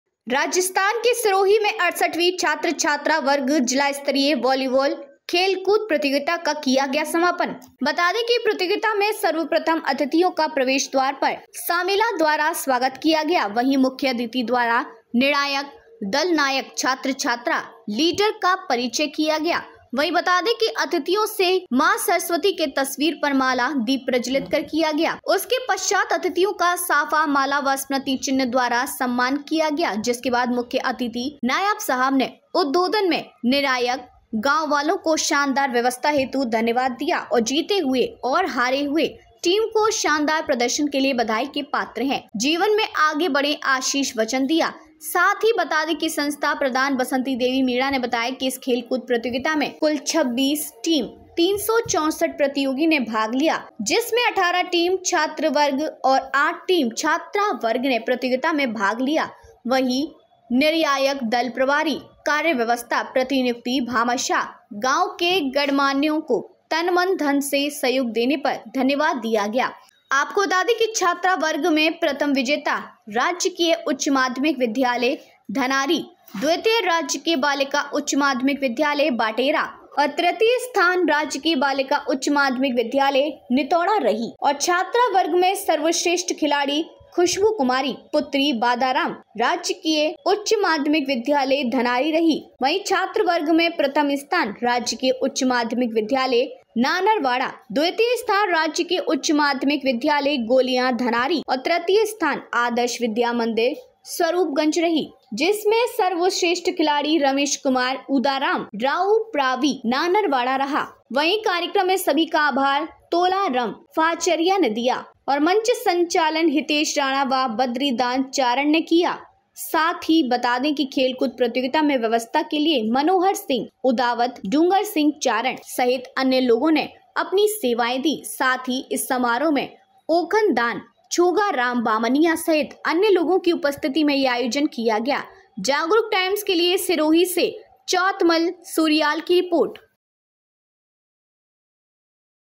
Sirohi News: 68वीं छात्र छात्रा वर्ग जिला स्तरीय खेलकूद Volleyball प्रतियोगिता का हुआ समापन
राजस्थान के सिरोही में 68वीं छात्र छात्रा वर्ग जिला स्तरीय बॉलीबाल खेलकूद प्रतियोगिता का  किया गया समापन।
बता दे प्रतियोगिता में सर्वप्रथम अतिथियों का प्रवेश द्वार पर सामेला द्वारा स्वागत किया गया।

वही मुख्य अतिथि द्वारा निर्णायक, दलनायक, छात्र छात्रा लीडर का परिचय किया गया। वही बता दे अतिथिओ से मां सरस्वती के तस्वीर पर माला, दीप प्रज्ज्वजित किया गया। उसके पश्चात अतिथियों का साफा, माला व स्मृति चिन्ह द्वारा सम्मान किया गया। जिसके बाद मुख्य अतिथि नायब साहब ने उद्बोधन मे निर्यायक, गांव वालों को शानदार व्यवस्था हेतु धन्यवाद दिया और जीते हुए और हारे हुए टीम को शानदार प्रदर्शन के लिए बधाई के पात्र है जीवन में आगे बढे आशीष वचन दिया।